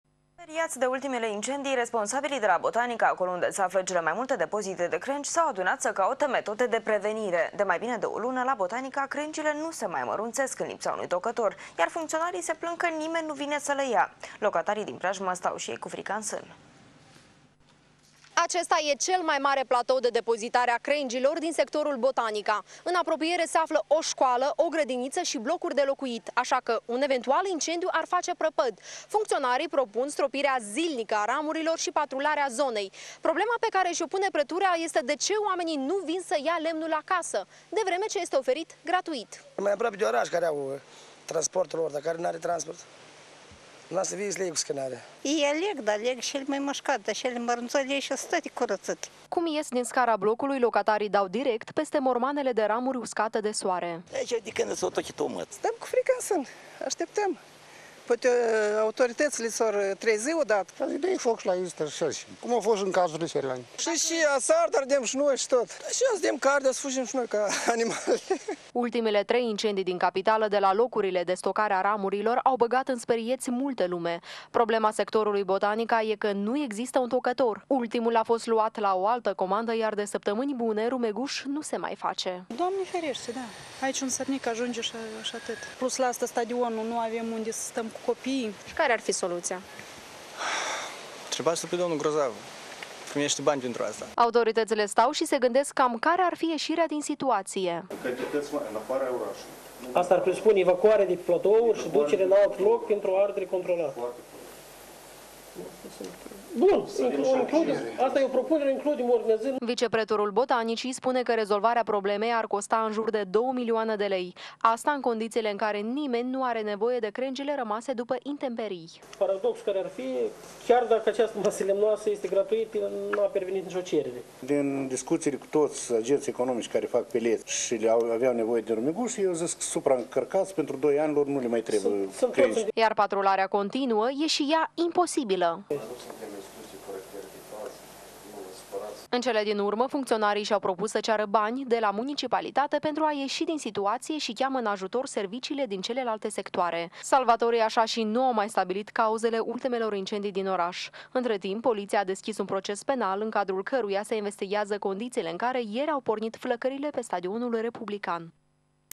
Speriati de recentele incendii, autoritatile cauta metode de prevenire - Pro Tv Chisinau
Speriati de recentele incendii, responsabilii de la Botanica, acolo unde se afla cele mai multe depozite de crengi, s-au adunat sa caute metode de prevenire: "Stam cu frica in san, o sa ardem si noi si tot"